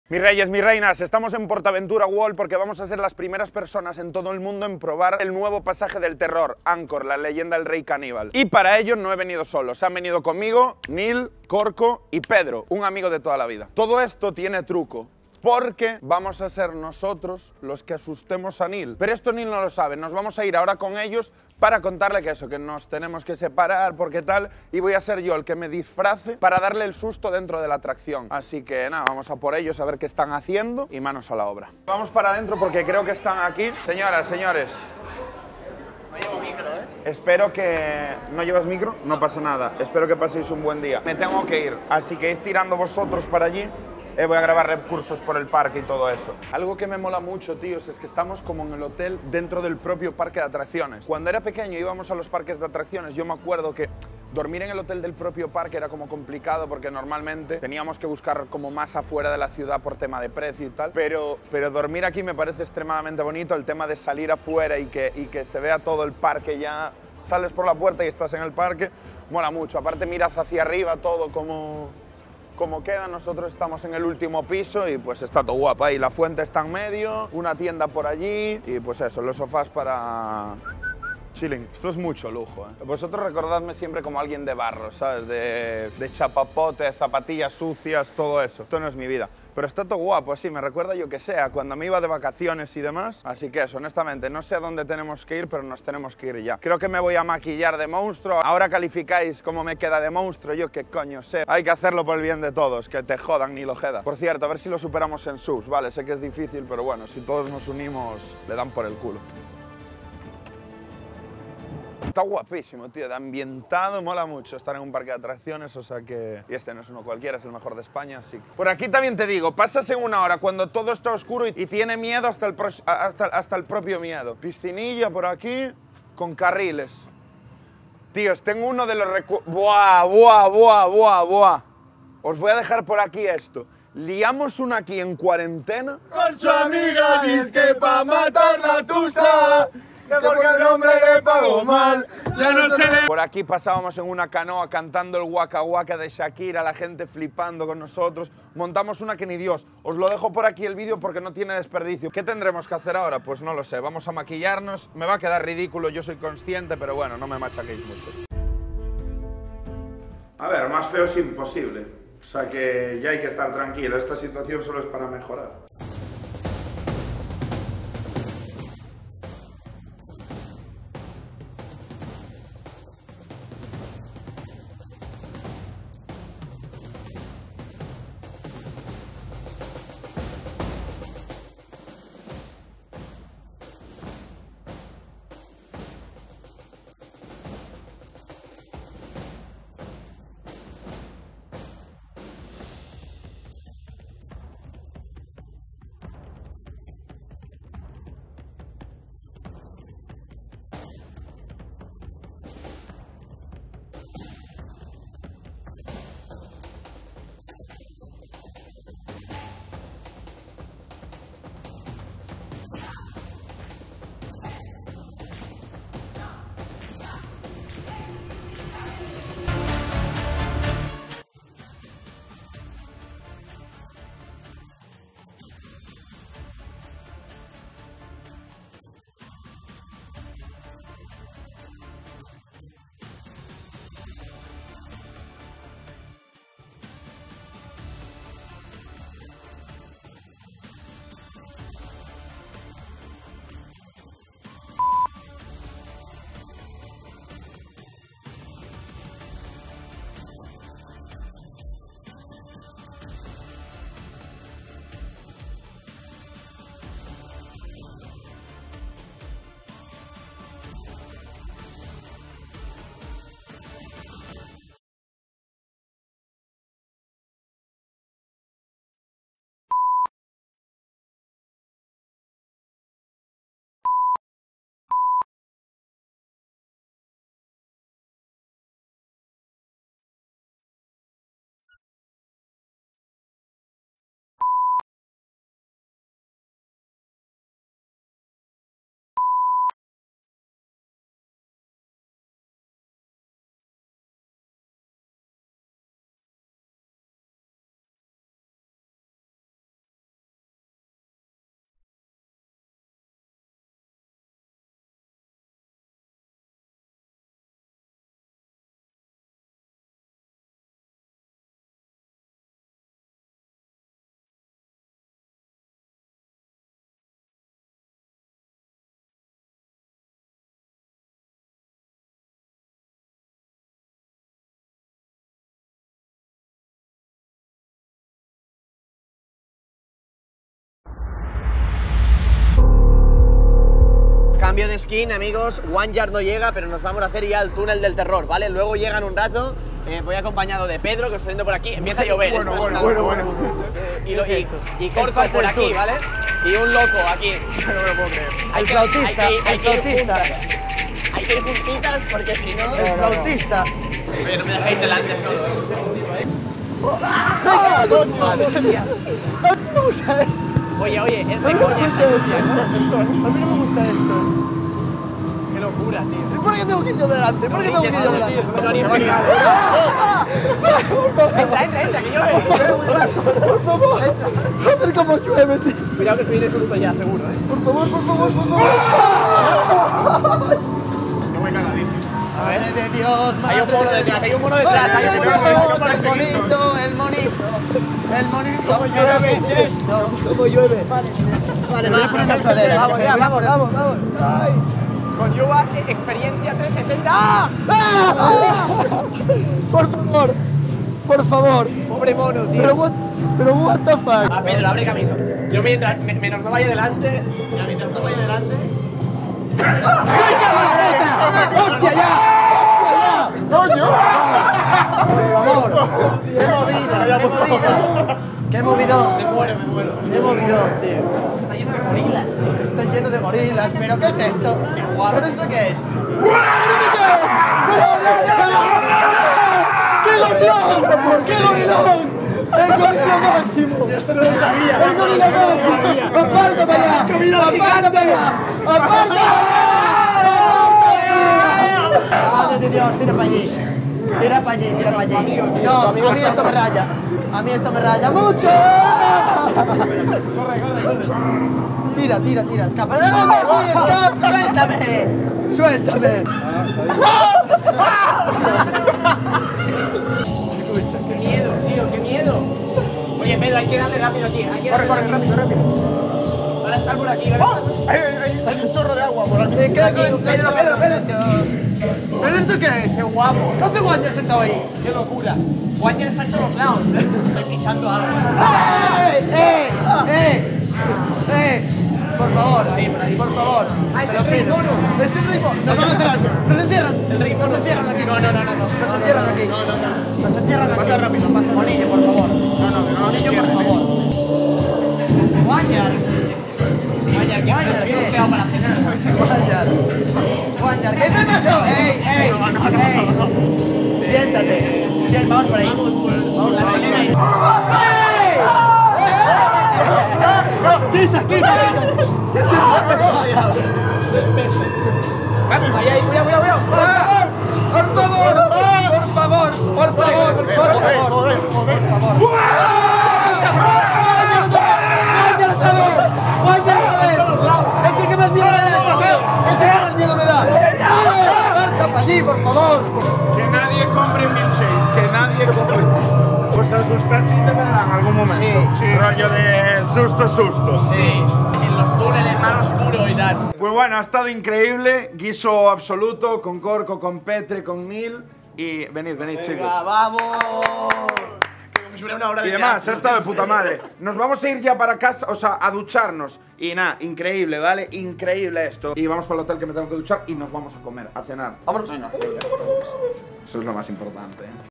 Mis amigos NO me HABLARÁN MÁS después de ESTO
Nos adentramos en el nuevo pasaje del terror Angkor, la Leyenda del Rey Caníbal de PortAventura World